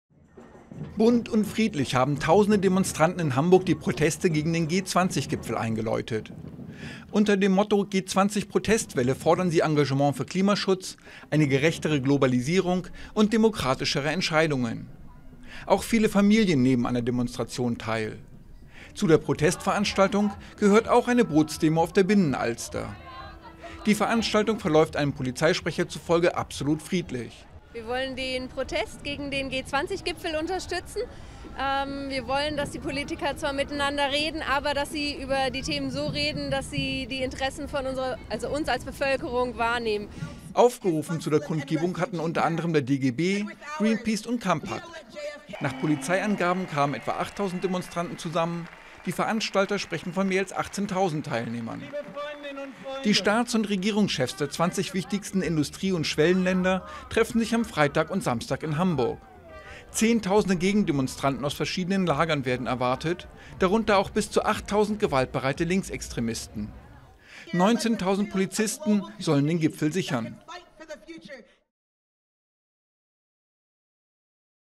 Erste G20-Demonstration verläuft friedlich
Bunt und friedlich haben tausende Demonstranten am Sonntag im Hamburg die Proteste gegen den G20-Gipfel eingeläutet: Unter dem Motto "G20 Protestwelle" forderten sie in der Innenstadt und auf der Binnenalster mehr Engagement für Klimaschutz, eine gerechtere Globalisierung und demokratischere Entscheidungen.